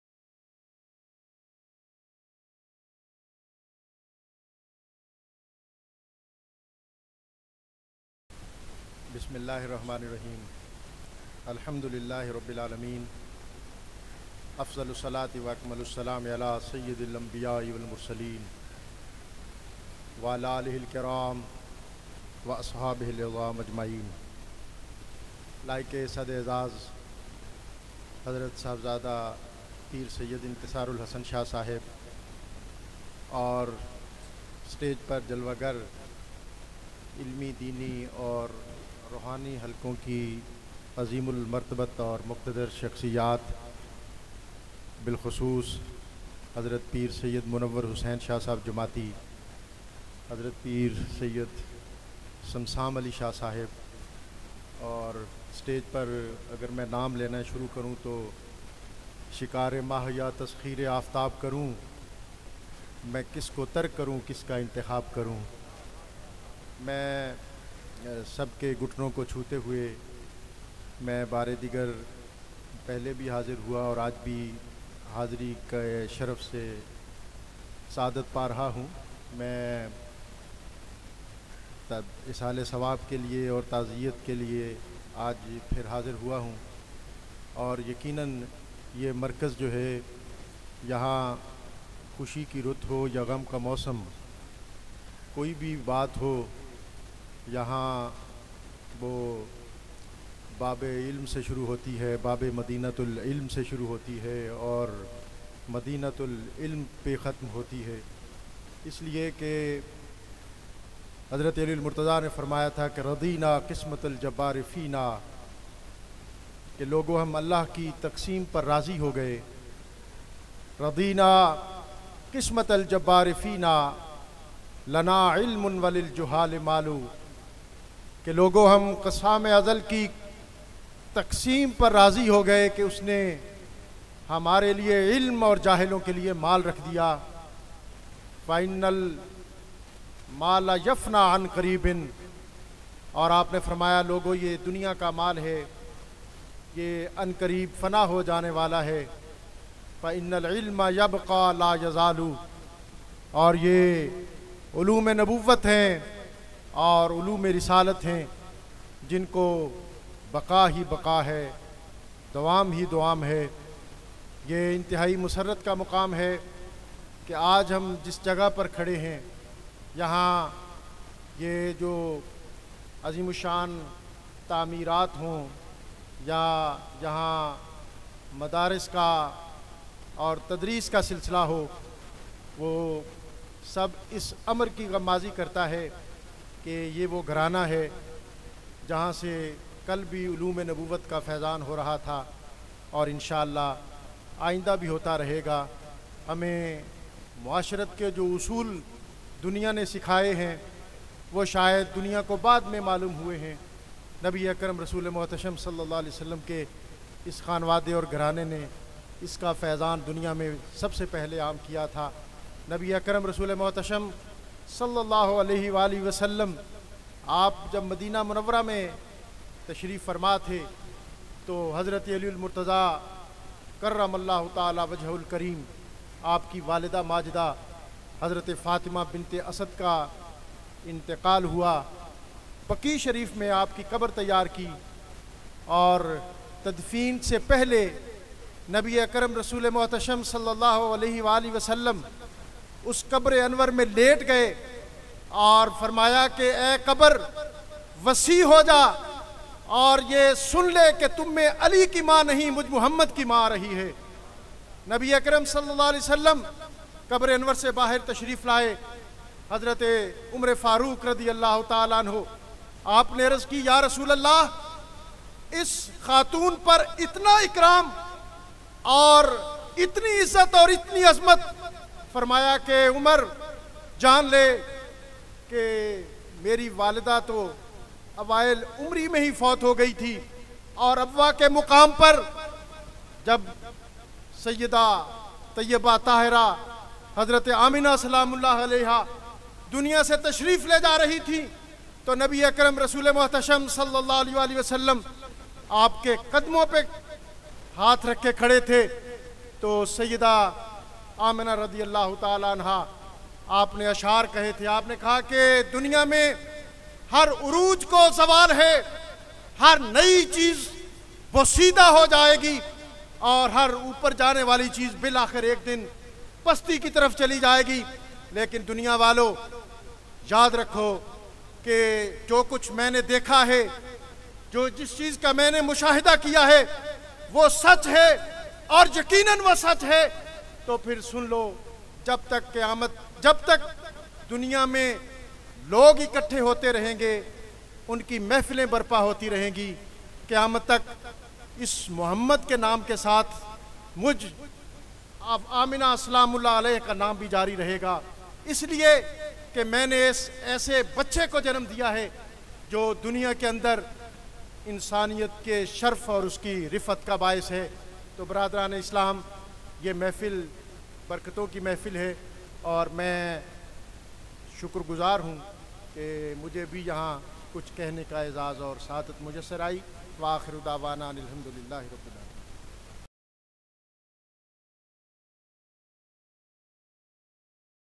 Khitab Syed Tahir Raza Shah Khatam Chehlum Sharif Aapa G Haji Sahiba (rehmatullah alaih) 2018
Khatam e Chehlum Sharif (Walida Majida) Pir Syed Intisar-ul-Hassan Shah Sahib Darbar Ghaus-e-Yagana Chally Sharif 14 January 2018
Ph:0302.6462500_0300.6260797